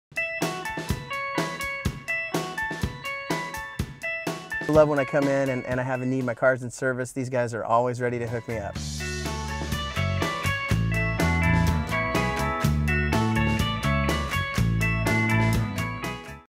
Great Customer Service At Campbell Nelson
Customer Paul Moi talks about the great service at Campbell Nelson. Campbell Nelson is an automotive dealership located in Edmonds Washington serving the Seattle area with Jetta, Passat, CC, Tiguan Volkswagen as well as Altima, Murano, Sentra, Titan and all the new and used Nissans. Meet our customers. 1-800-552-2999